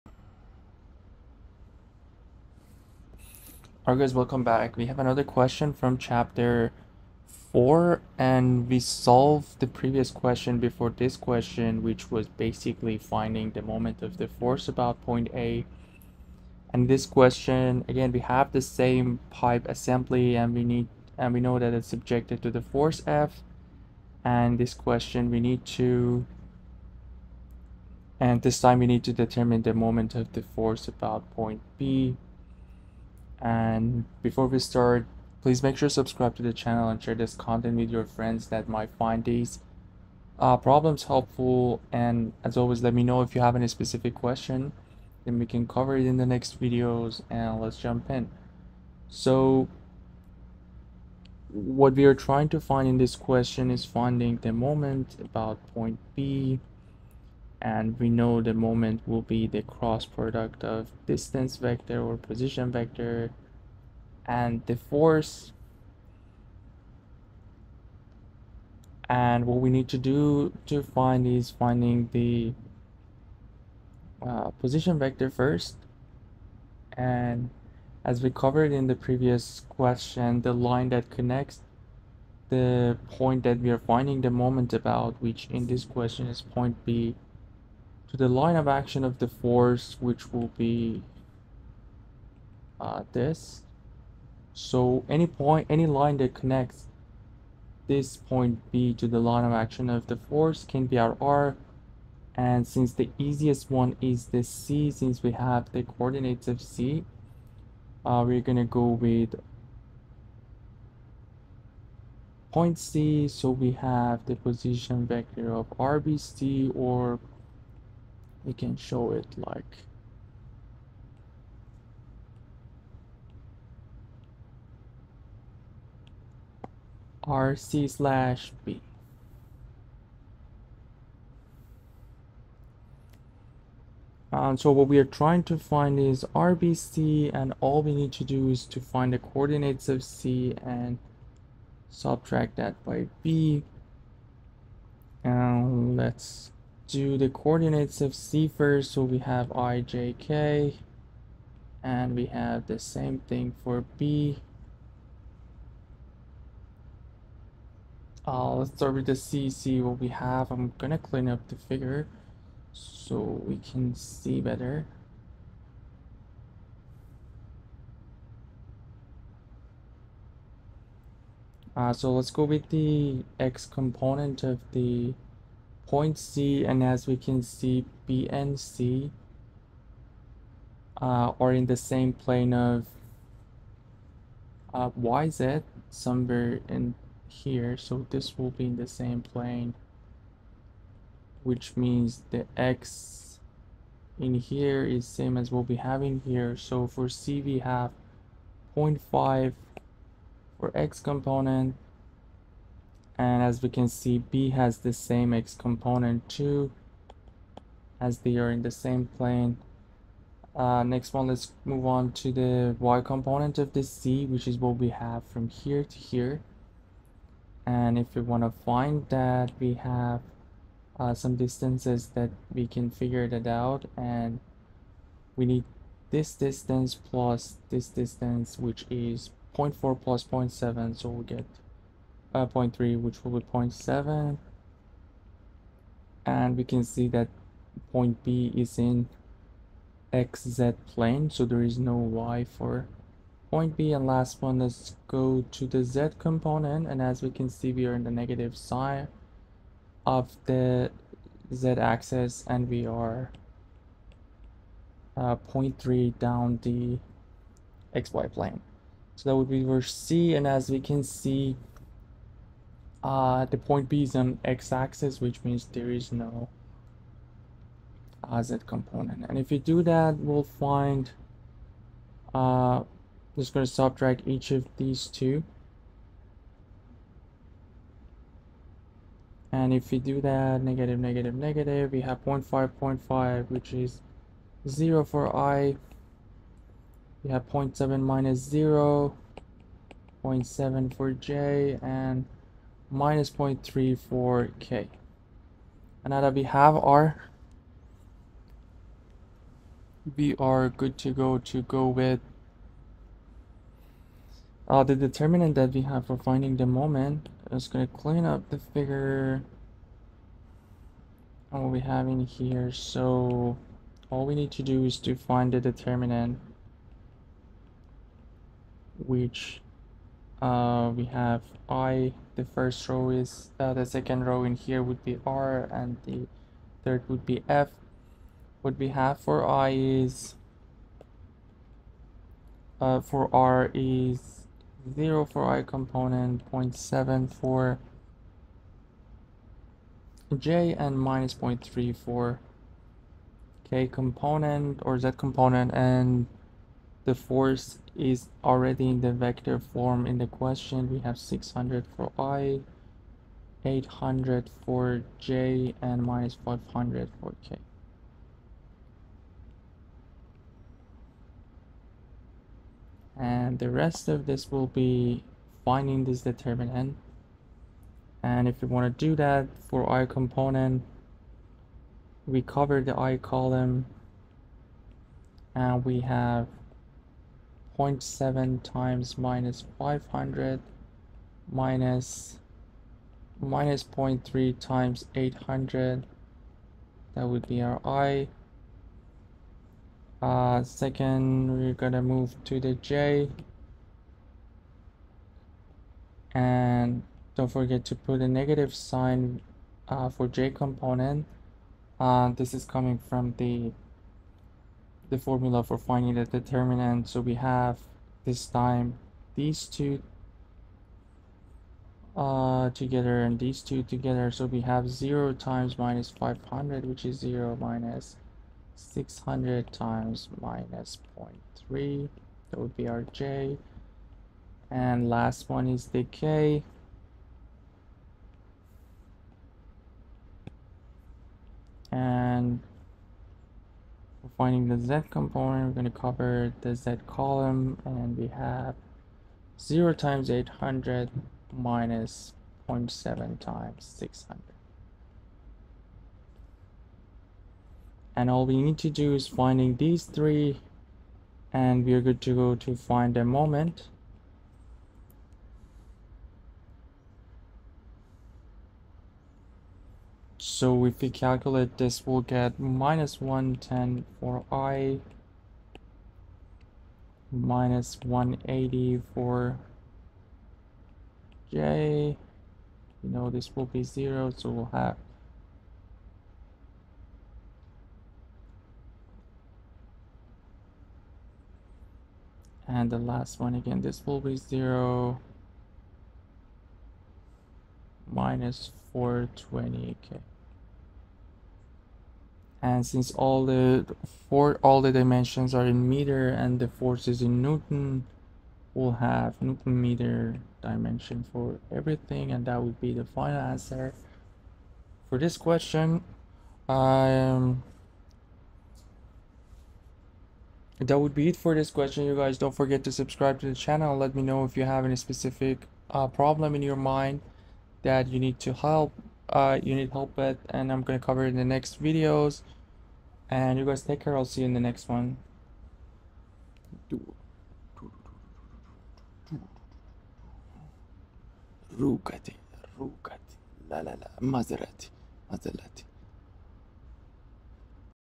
Problem 4.33 | Engineering Mechanics Statics
Solved Problem 4.33 | Engineering Mechanics-Statics-14th edition-R.C. Hibbeler:

The pipe assembly is subjected to the force of F = {600i + 800j - 500k} N. Determine the moment of this force about point A.

00:00 Intro
01:23 Finding of R
05:00 Finding of Moment
07:23 Final answer